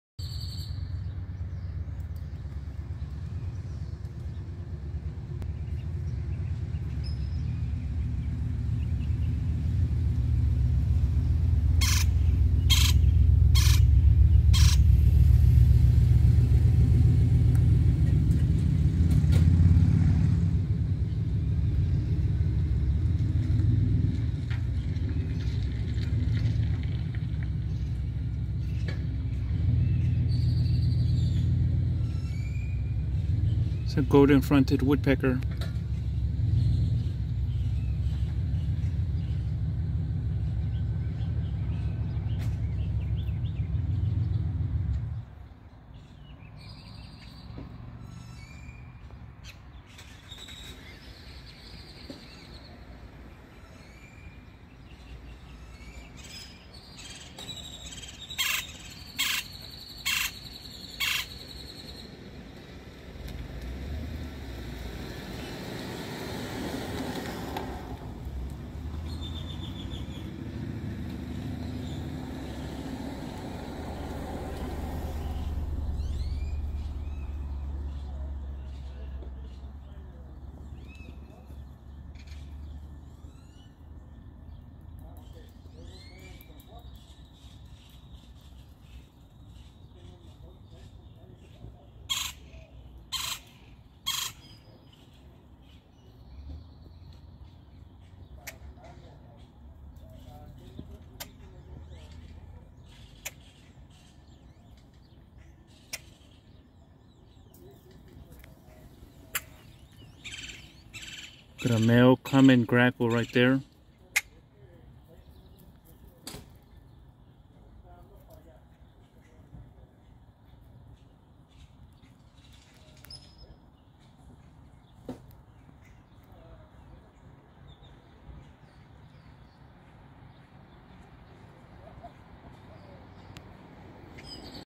Gibson Park in Weslaco, Texas. Golden-Fronted woodpecker and Common Grackel.
3.25.22. Friday

Gibson Park in Weslaco, Texas. not the best video of a Golden-Fronted woodpecker outhere.  also,  a male Common Grackel.